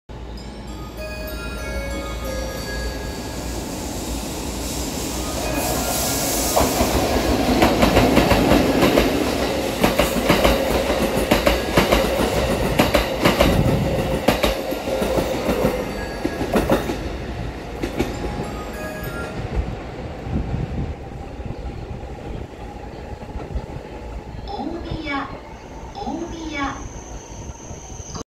ジョイント音最高！【速度が早すぎる！】国鉄EF65 2080[新]+グリーン車4両(F-21編成、F-22編成用)が大宮駅10番線を高速通過するシーン(9771レ・甲73) 2022.8.2
先週の金曜日は諸事情により投稿をお休みさせて頂きました
ご心配おかけして誠に申し訳ありませんでした🙏
本日スカレンジのグリーン車の甲種輸送がありました。スジ公開の時間書かれた前に余裕を持って大宮駅に着いたので再び甲種輸送に遭遇することができて本当に嬉しい🥰
しかも今回は前回とは違って高速通過でした！！！！
2回連続で甲種輸送に遭遇したから連続で甲種輸送に遭遇したのは人生で初めてかも🥳
ちなみに今日の大宮駅11番ホームに同業者が沢山いたのでめっちゃ驚きました。